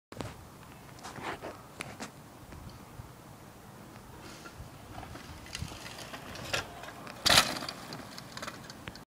Mountain bike step down jump
Its a jump off a root at a school by my house